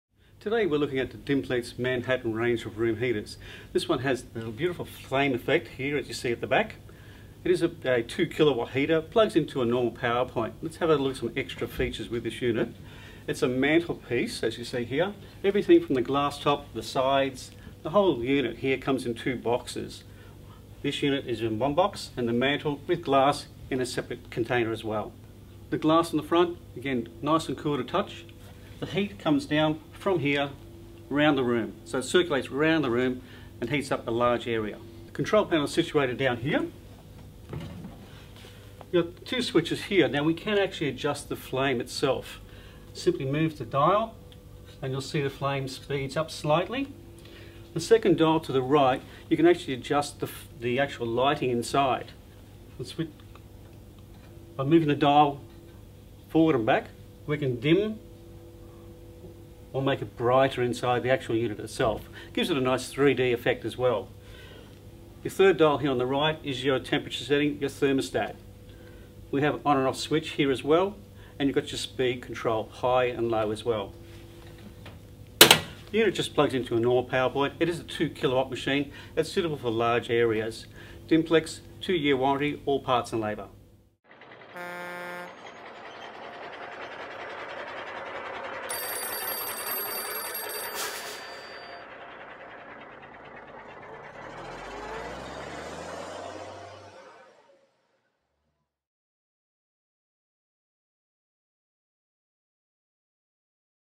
Expert demonstrates the Dimplex Manhattan electric heater with 3D flame effect - Appliances Online
The 2kW Dimplex Manhattan electric fan heater comes with patented 3D flame effect technology.

Cool to touch glass on the front of the heater means it is safe in the home, and you can operate the unit using a three stage remote control.

Two switches allow you to control the effect of the flame and the lighting inside the heater.

A third thermostat dial means you can control the heat coming out of the electric fire.

The full mantlepiece, including the glass top and black and white surrounds are included in the package.

The unit plugs into a normal power point, and no special installation or venting is required.